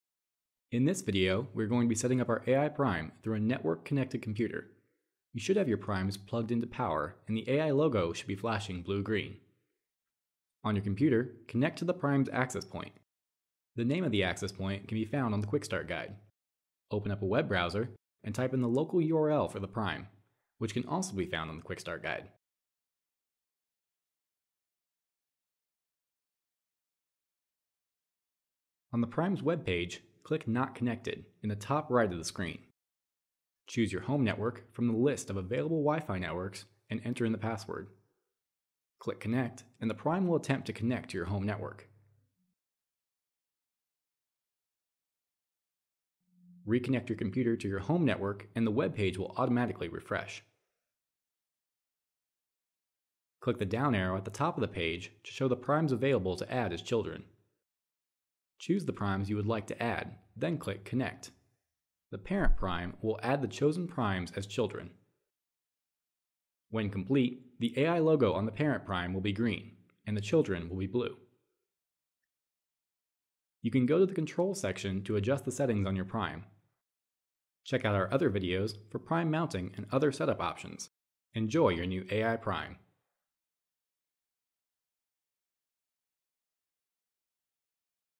AI Prime® - Web Setup
Setup your AI Prime® using a network-connected computer.